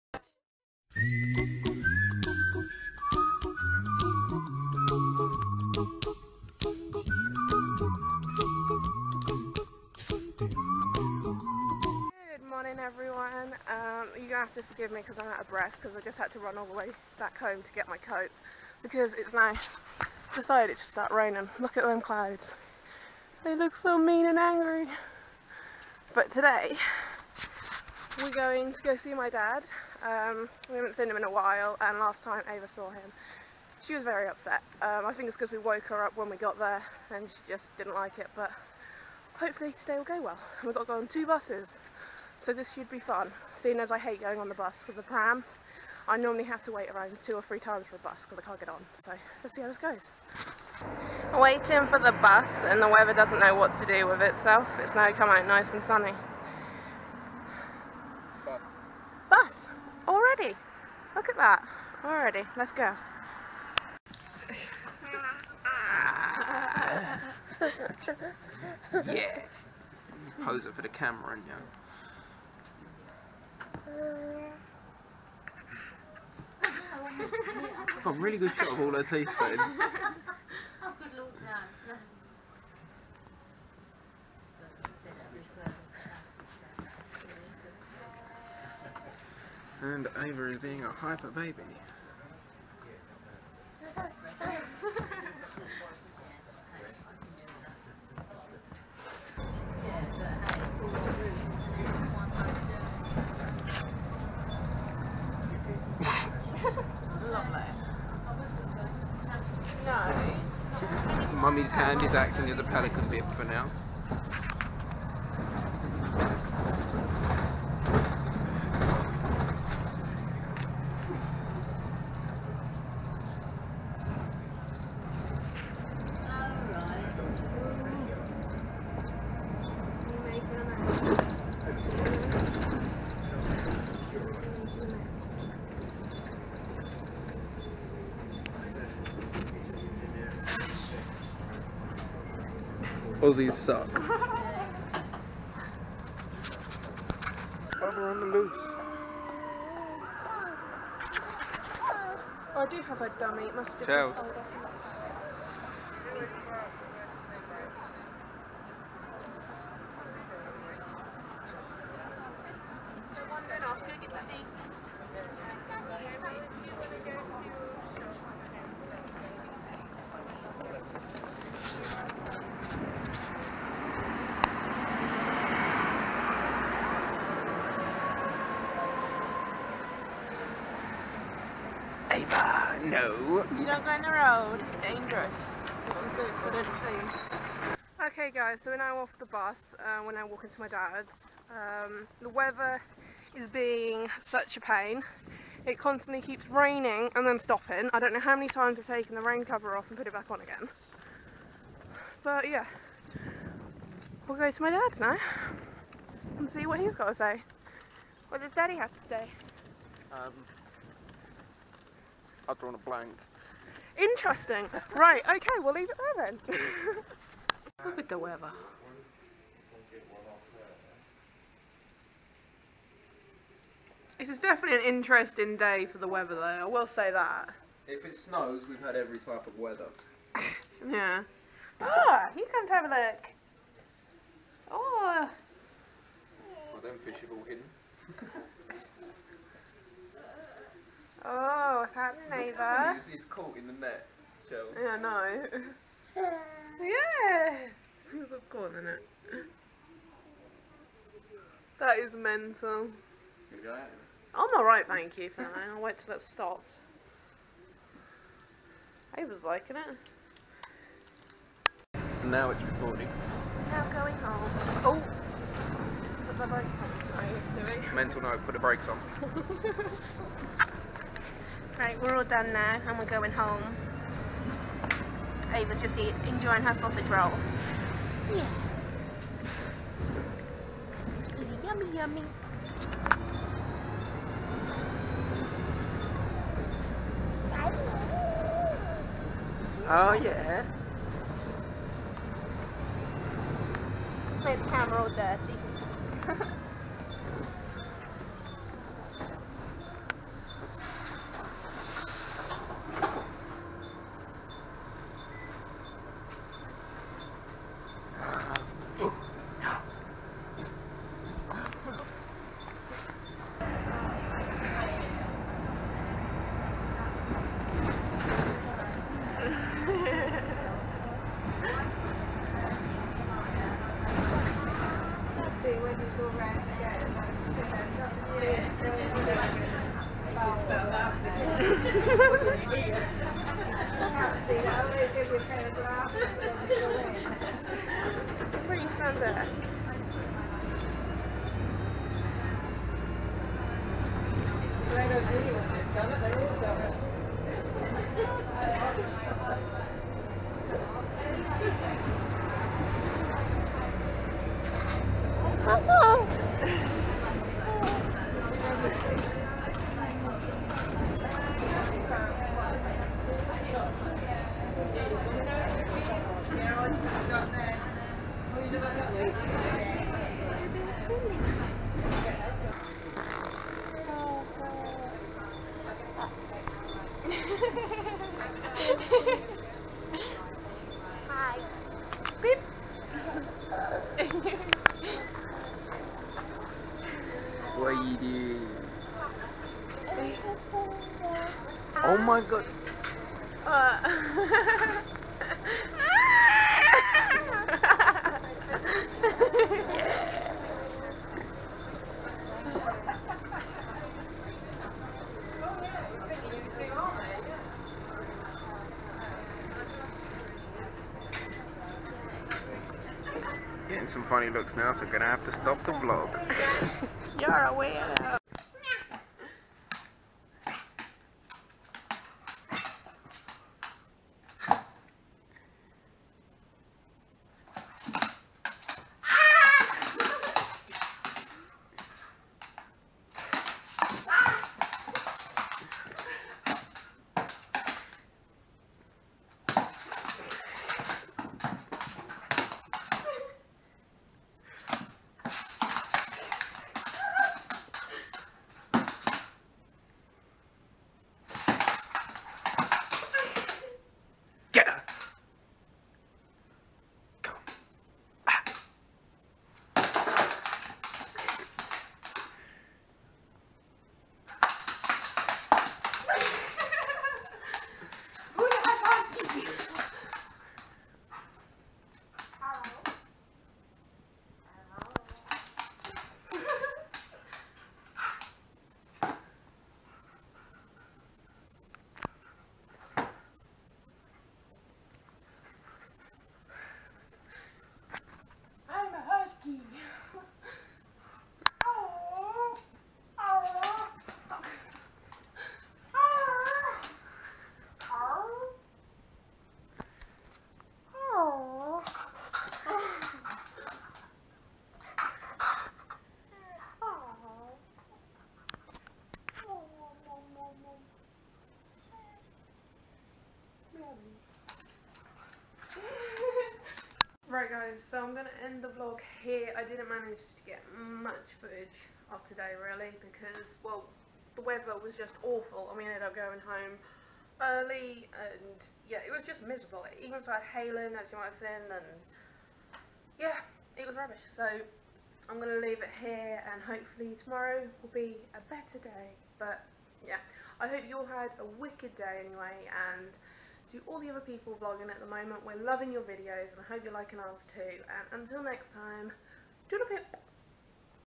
MUMMY'S TURNED INTO A HUSKY !?! vlog #9
19/05/2015 vlog  Ava makes friends on the bus :)